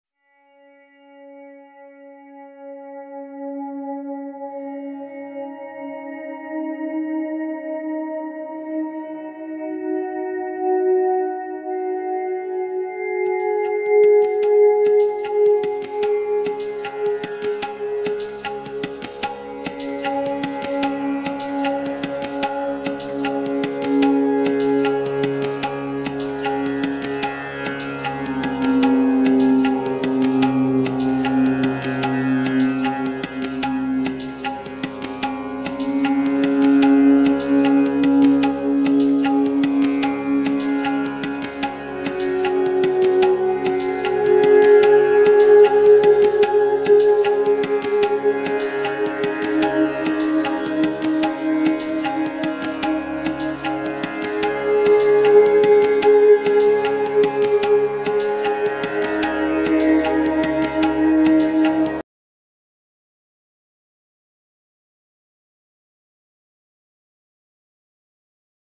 Nature , sea, Space beautiful scenary and music may help in relaxation and sleep.
Nature , sea, mountain, sunset and beautiful scenary and music may help in relaxation and sleep.